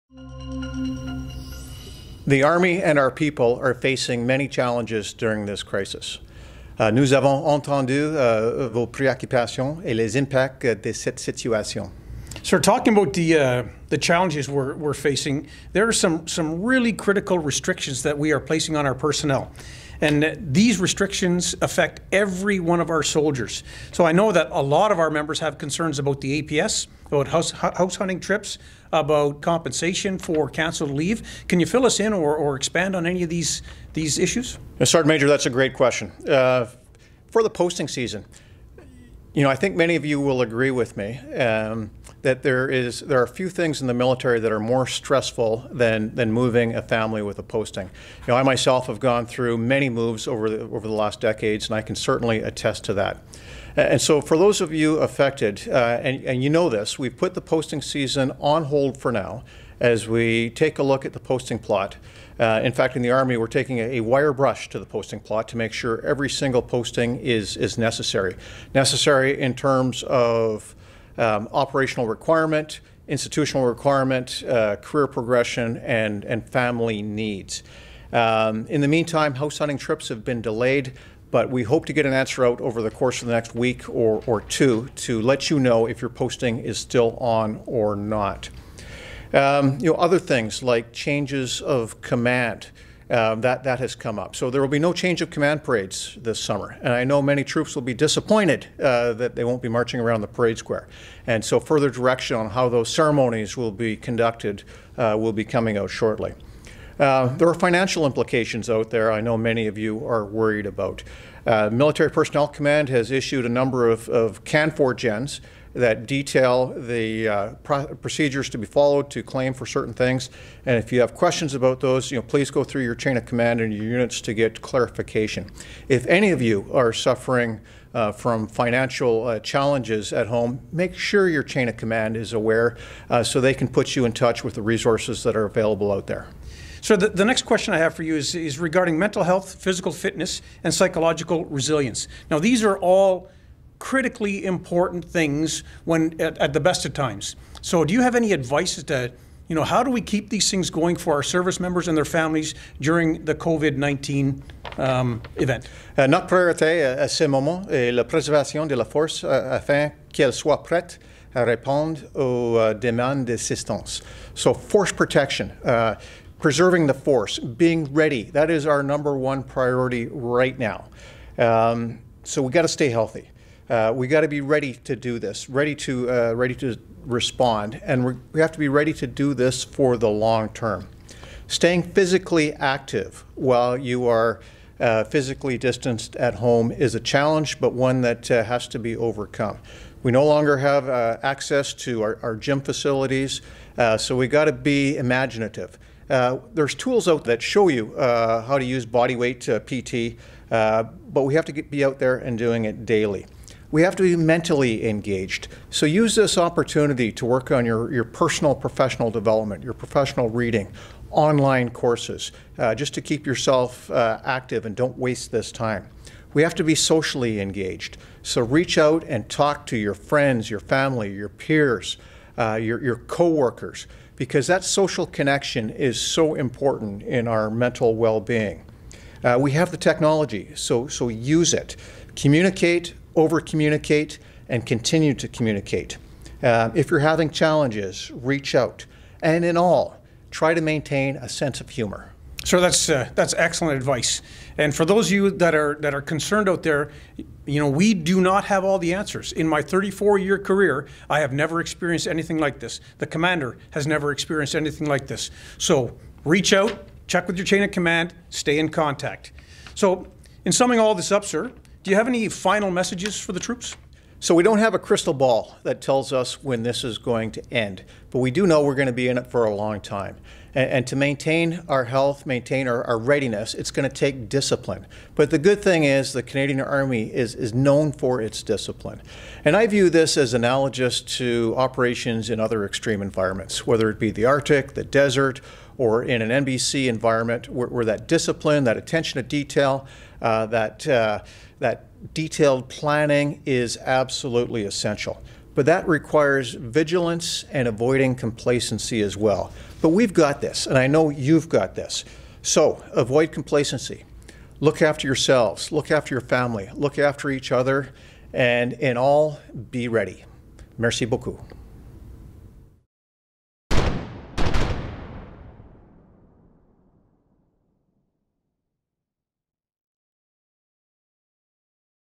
Message du Lgén Wayne Eyre et de l’Adjuc Stu Hartnell - le 14 avril 2020
Nouvelle discussion sur les changements importants mis en place en raison de la pandémie de COVID19, situation qui touche chacun de nos militaires et les membres de leurs familles. Merci à tous d’accepter de rester